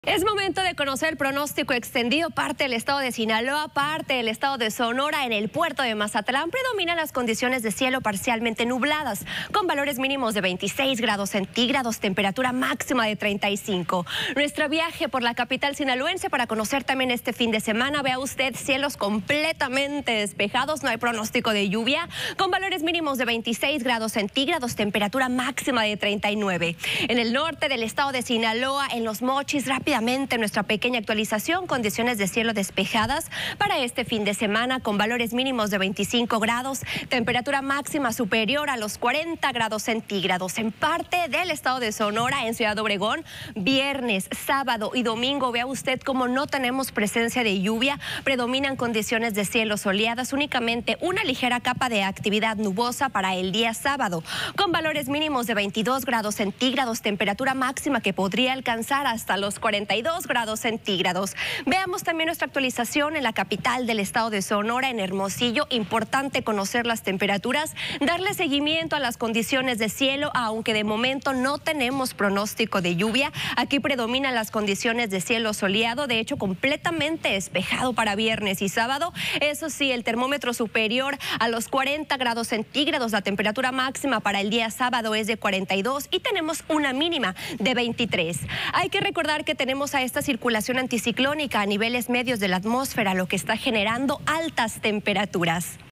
🌥️ ¡Sonora y Sinaloa hoy en un dilema meteorológico!
El cielo se pinta con nubes, pero el calor y la humedad se aferran. ¡Un día de contrastes que nos mantiene en vilo! ☁️🌞🌿